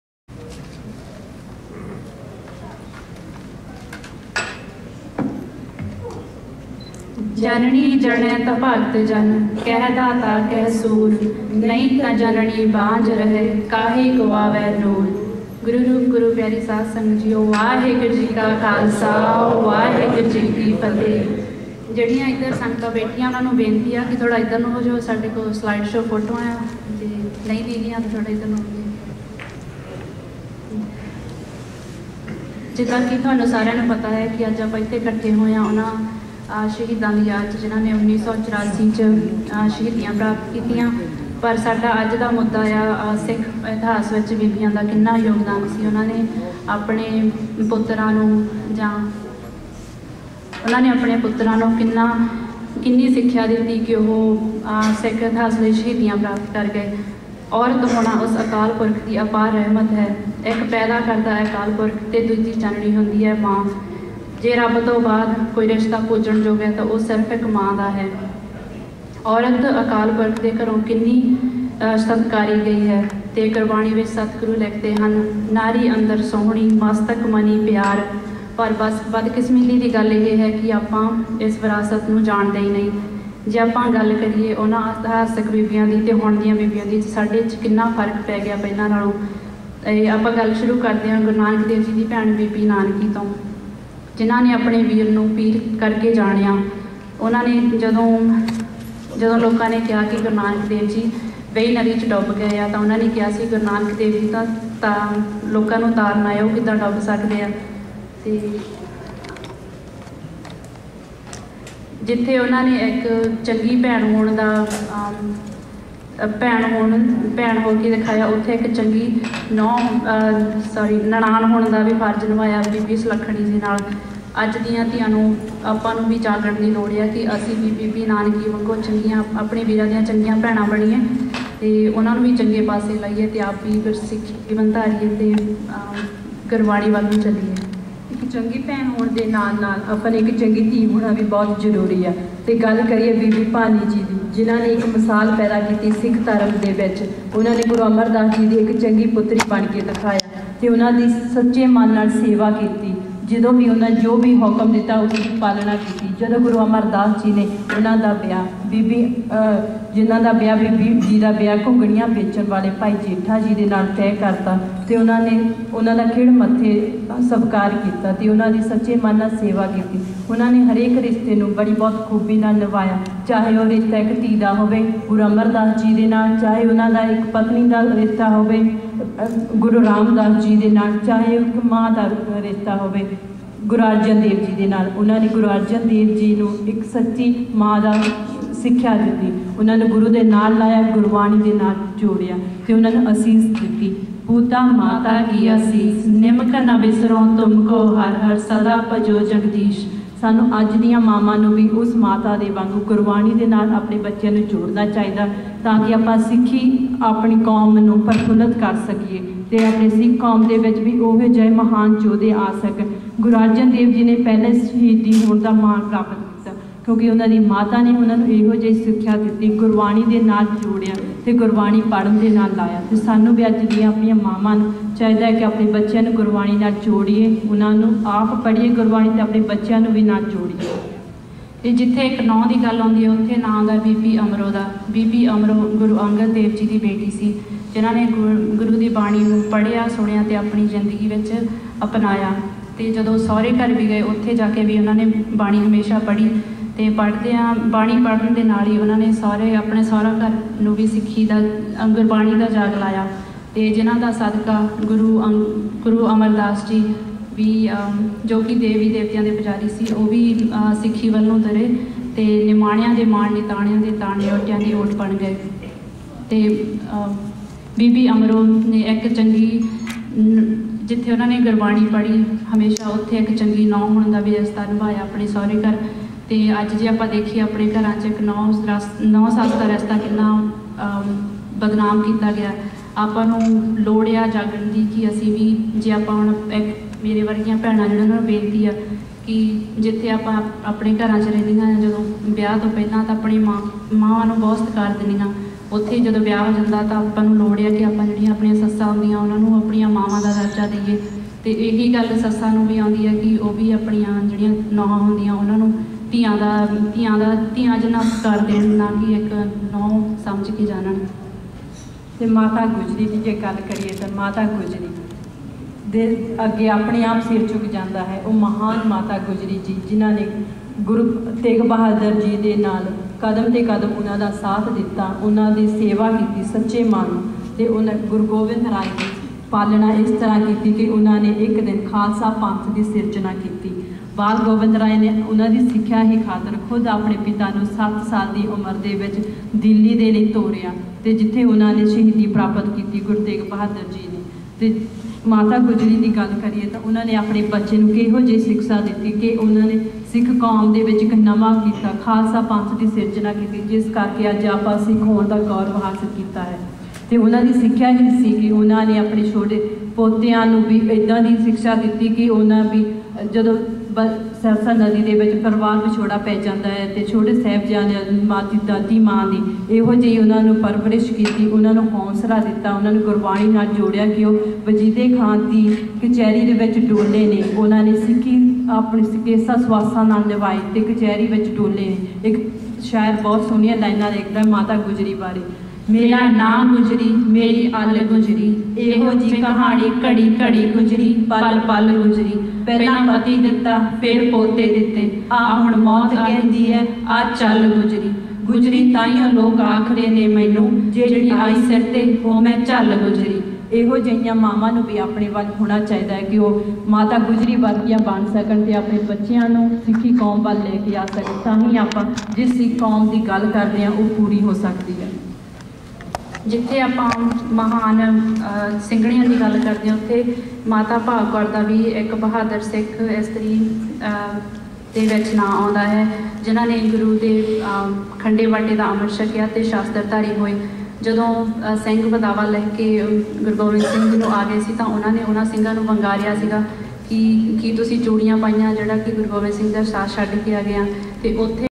Gurpreet Kaur & Amandeep Kaur Speech part 1.wmv
Gurpreet Kaur & Amandeep Kaur speech at Shaheedi smagam june 2011